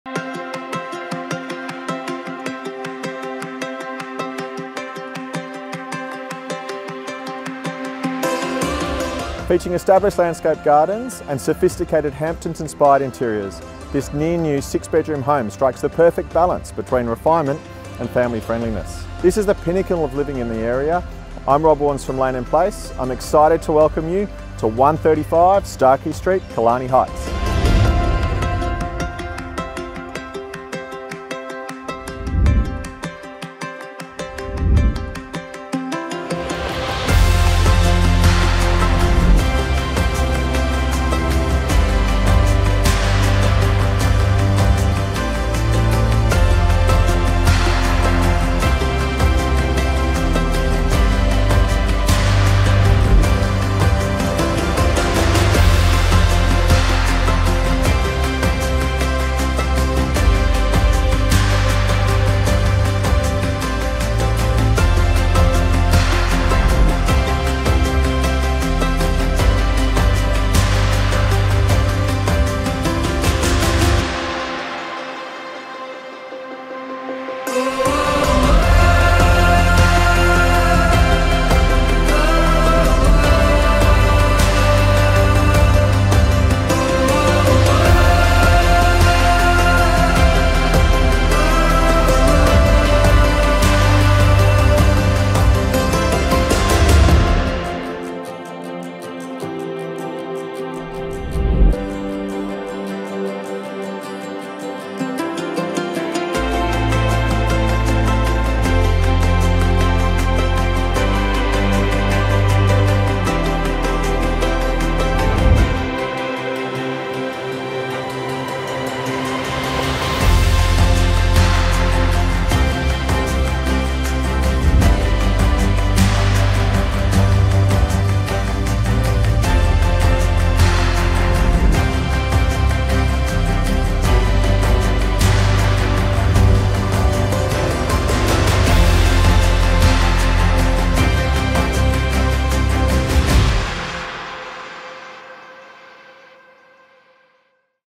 Expressions of Interest: 135 Starkey Street Killarney Heights
This stunning recently-completed (Dec 2021) residence offers unparalleled comfort and style. Featuring established landscaped gardens and sophisticated Hamptons-inspired interiors, this home strikes a perfect balance between refinement and family-friendliness. The attention to detail and high-quality finishes throughout the property make it a truly inspiring living space.

In addition to its aesthetics, the location offers proximity to some of Sydney's best schools, ensuring excellent educational opportunities for families. The transportation services available right at the doorstep make commuting and accessing the beaches, Chatswood and the city a breeze. Undoubtedly, this property presents a rare opportunity to acquire a magnificent home that epitomises the pinnacle of family living in the area.

Agent contacts:
Rob Warnes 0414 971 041
Julie Warnes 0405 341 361